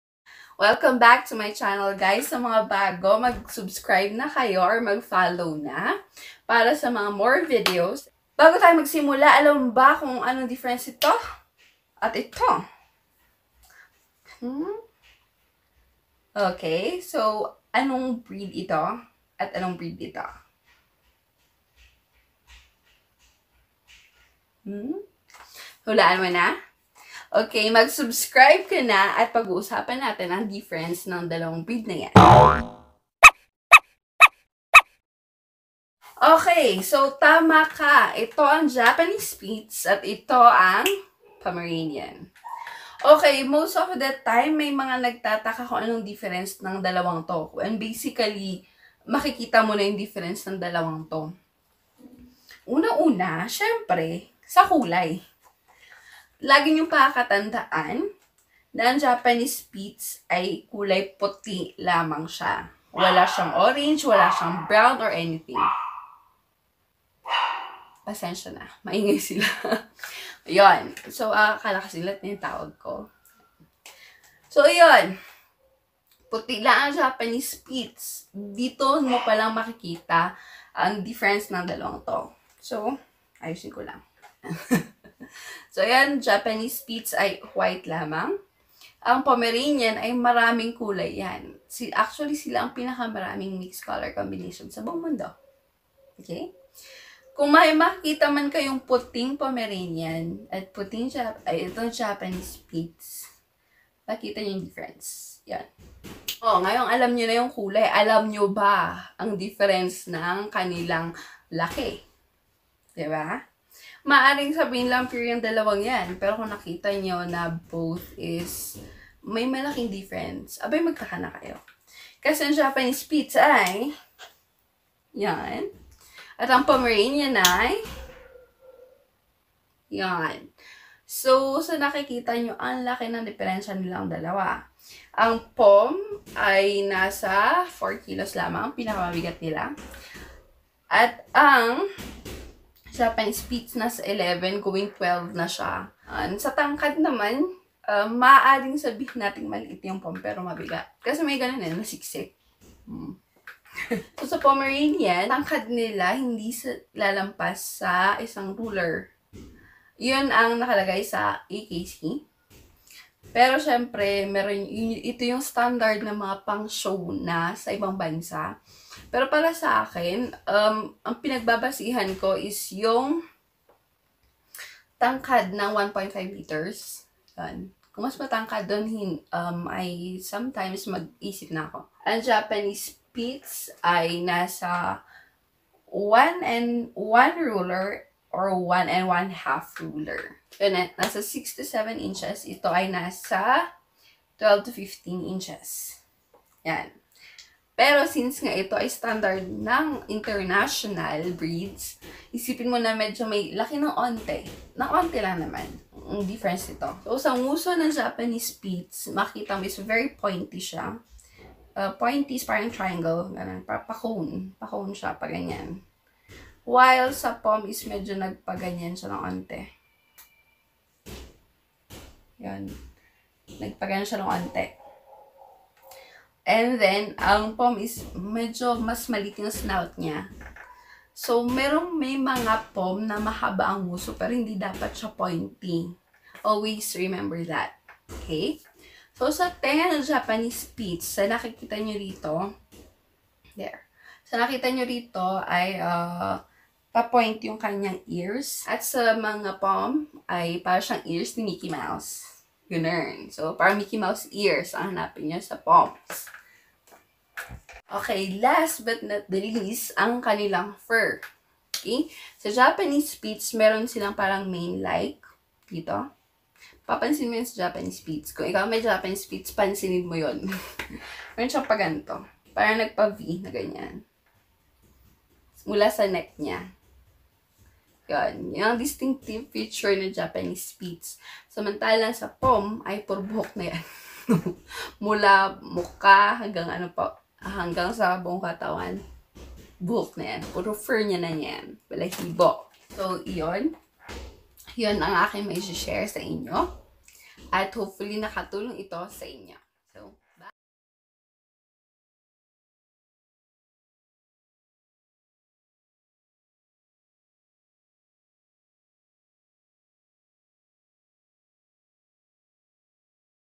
Ano ang Pinagkaiba ng Japanese Spitz at Pomeranian
and our tiktok account: @furrycategoryvlog